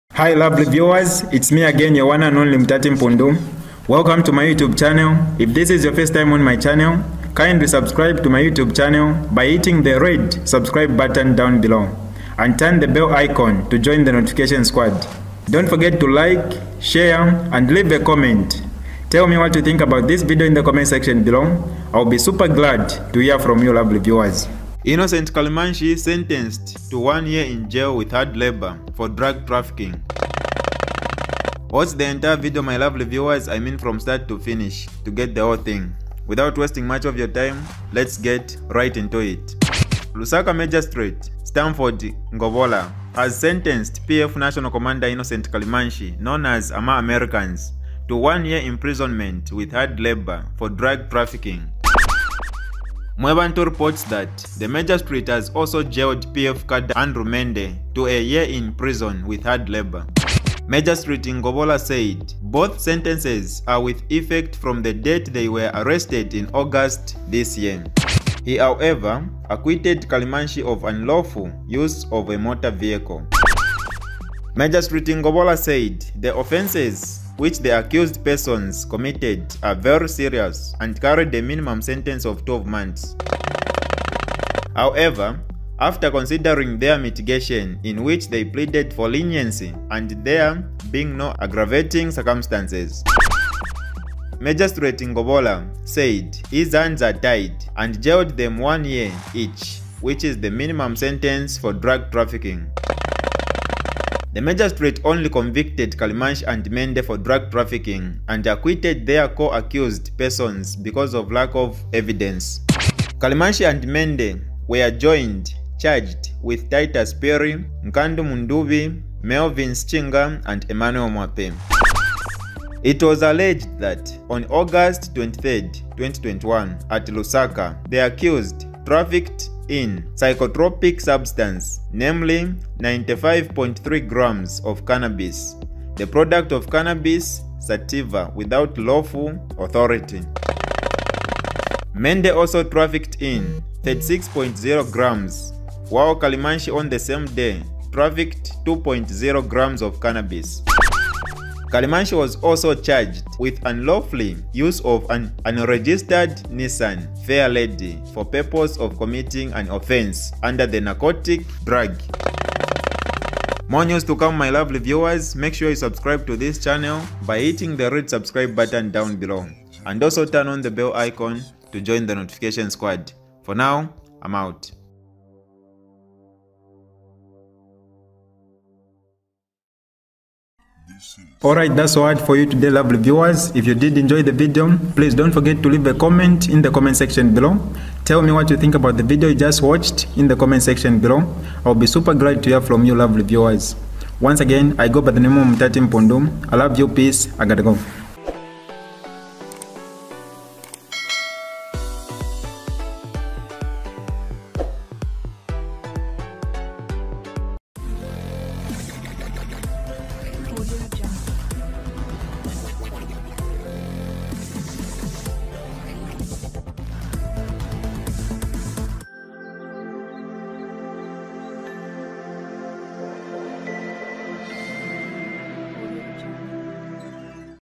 PF Cadre Innocent Kalimanshi Sentenced to 1 Year in Jail with Hard Labor for Drug Trafficking.......
PF Cadre Innocent Kalimanshi Sentenced to 1 Year in Jail with Hard Labor for Drug Trafficking (Must Watch)